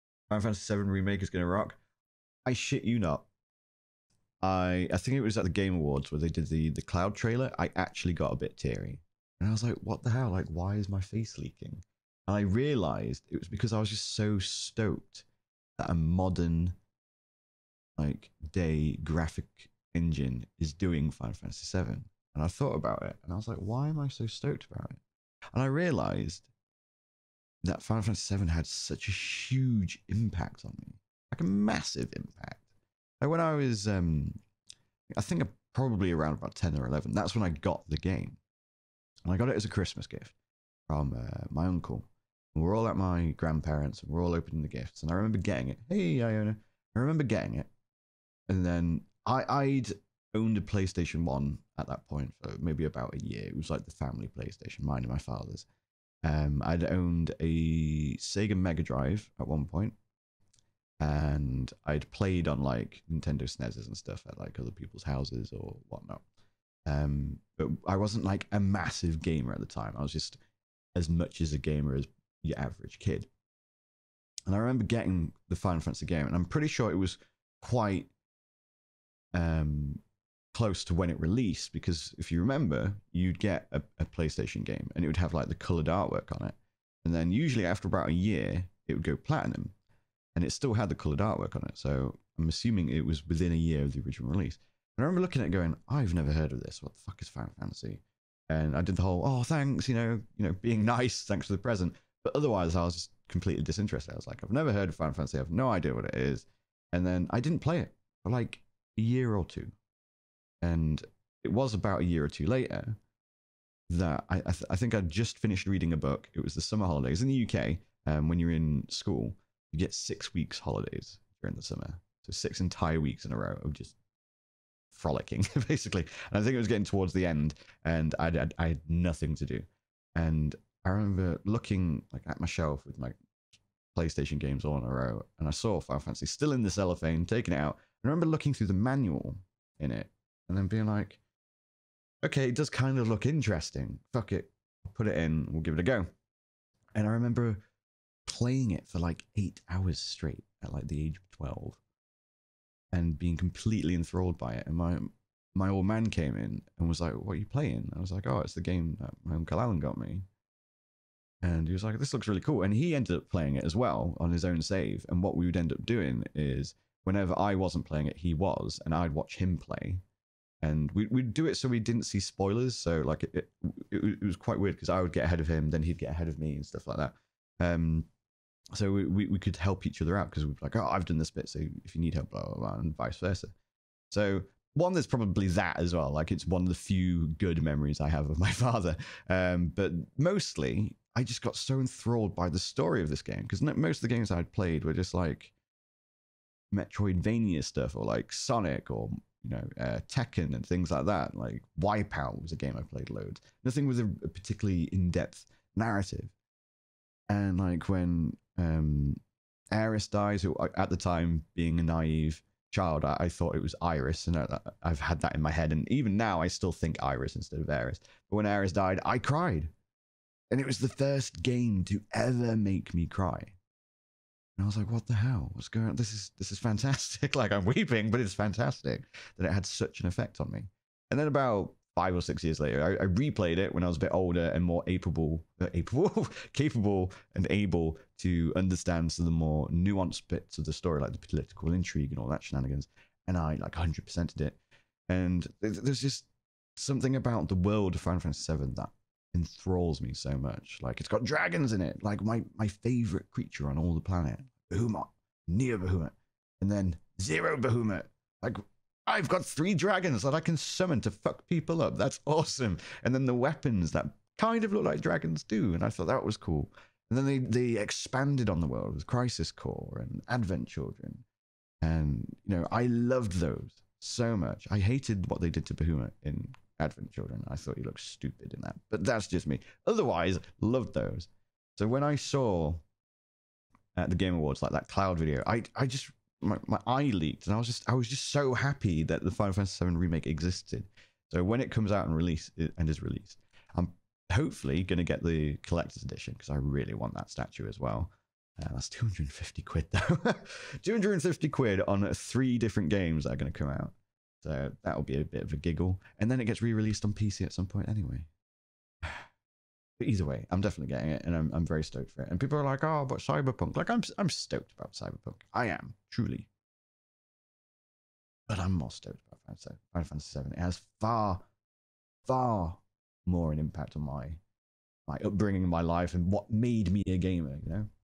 Why I am More Excited for Final Fantasy 7 Remake Than Other Games This Year
I get asked why I am more excited for FF7R than Cyberpunk 2077 or The Last of Us II a lot...

Story time! A highlight from my stream where I talk a little about my childhood, FF7 and why I am more excited for it than any other game releasing this year (possibly ever!)